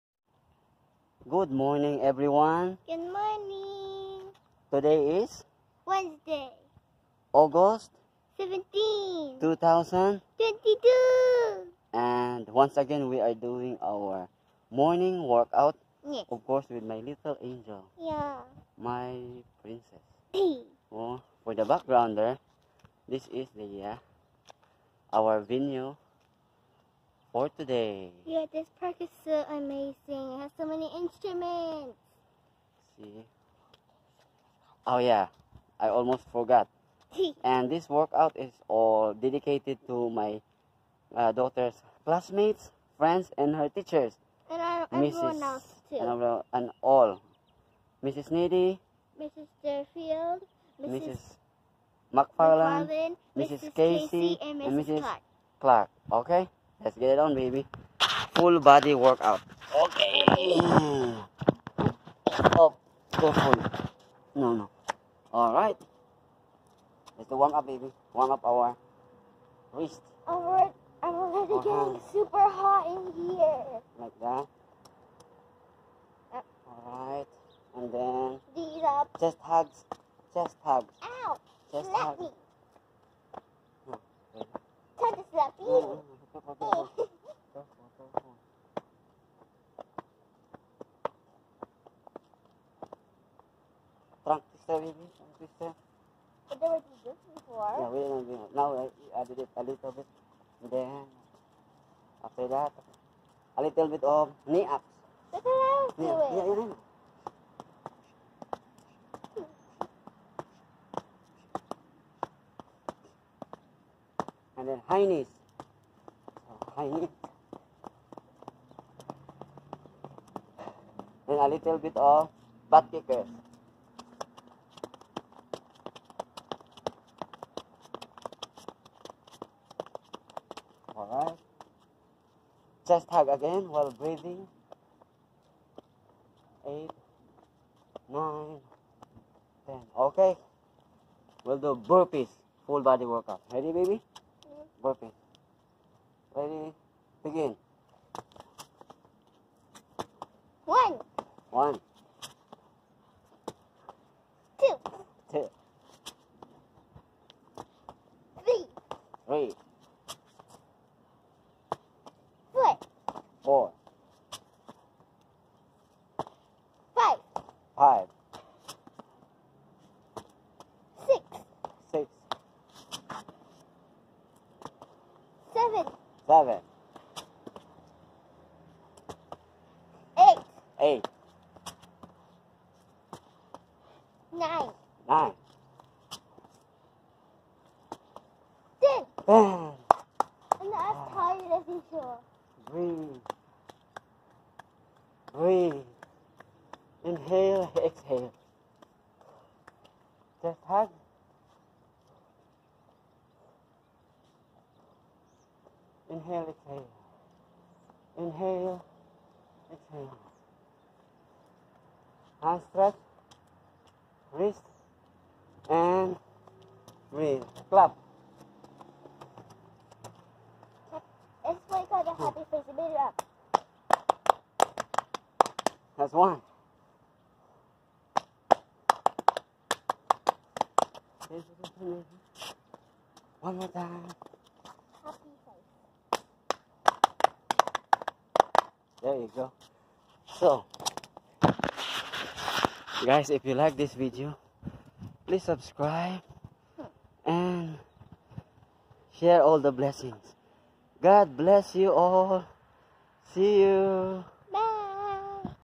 Wednesday Workout 'BURPEES' / August 17, 2022
Morning 🌄 exercise 👊🏼 with my little angel 20220817 🇺🇸